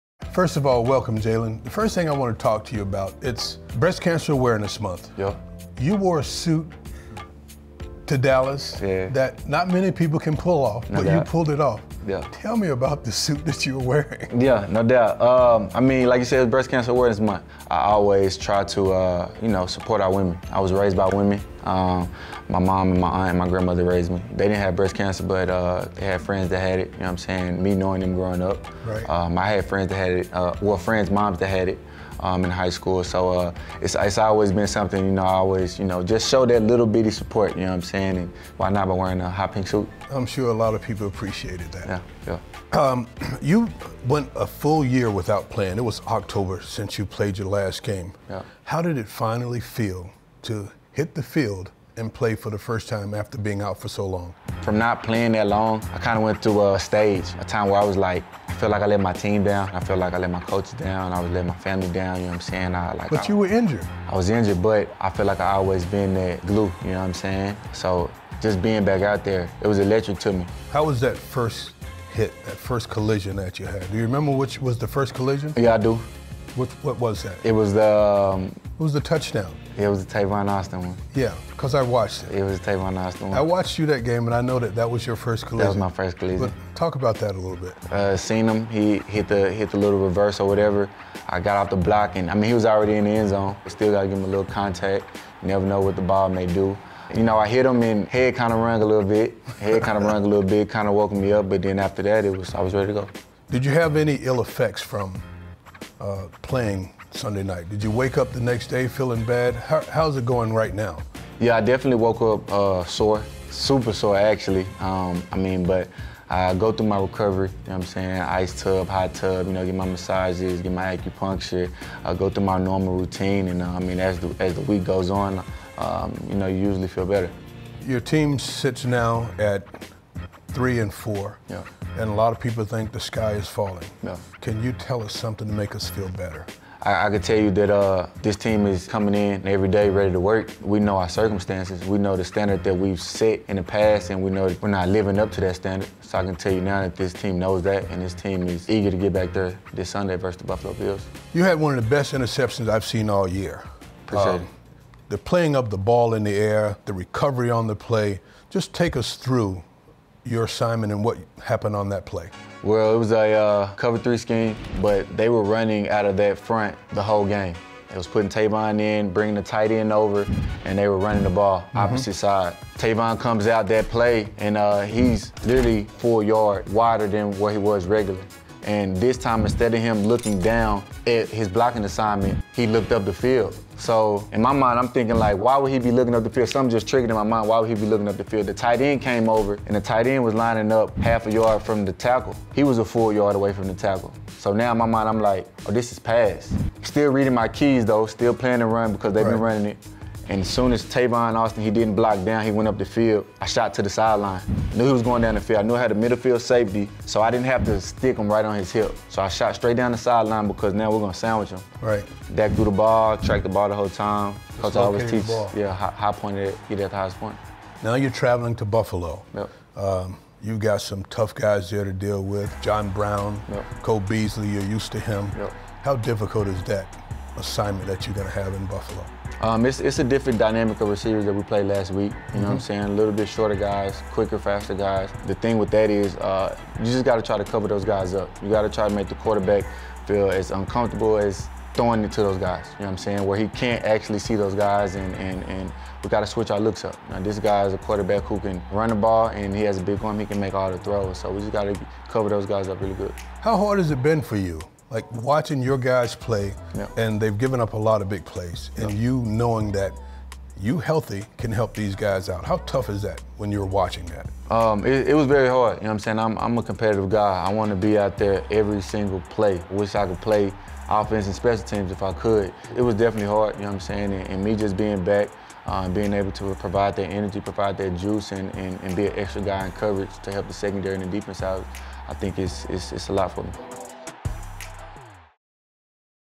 Jalen Mills Discusses His Long Road Back to the Field & More | Eagles One-On-One
Philadelphia Eagles cornerback Jalen Mills sits down with Eagles legend Mike Quick to discuss his long road back to the field and more.